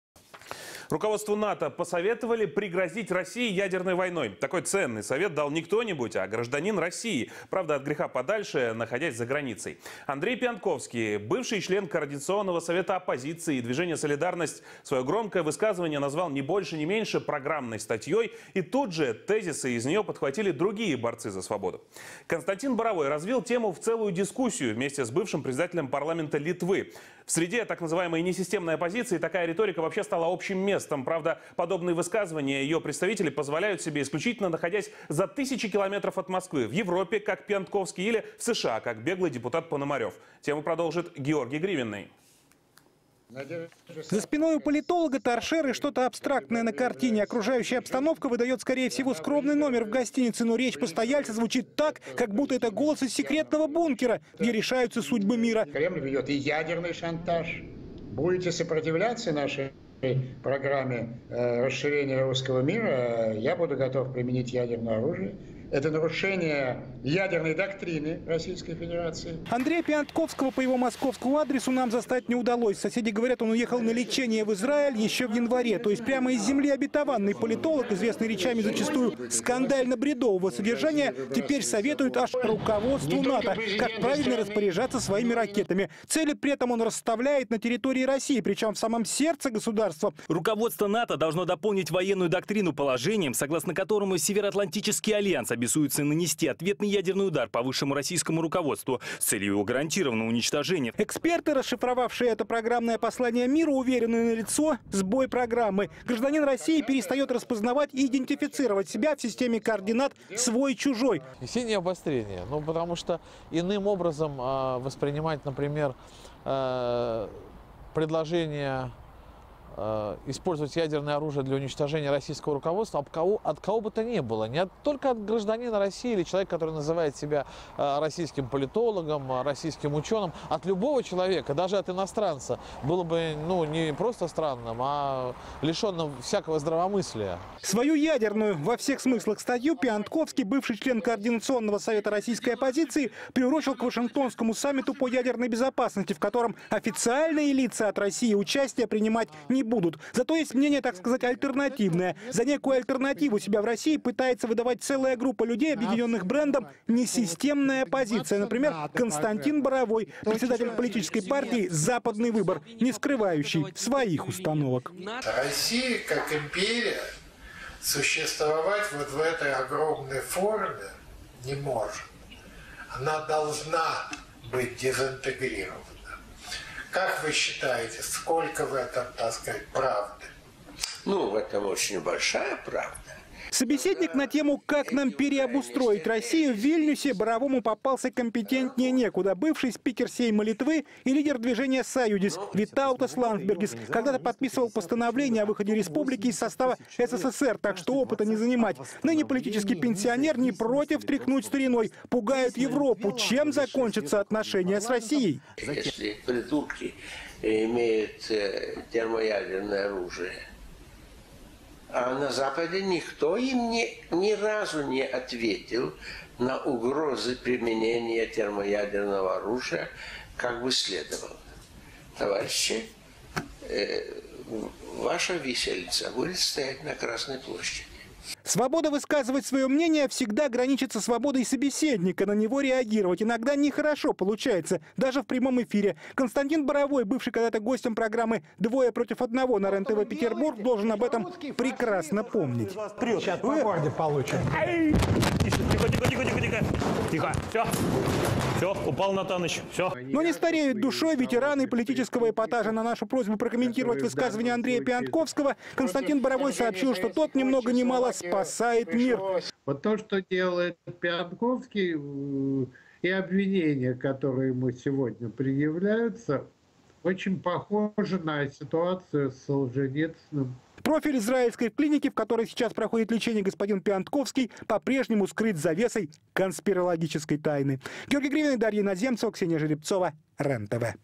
Эксперты объяснили слова Пионтковского весенним обострением
А так же не забывайте подписываться на наш канал на YouTube.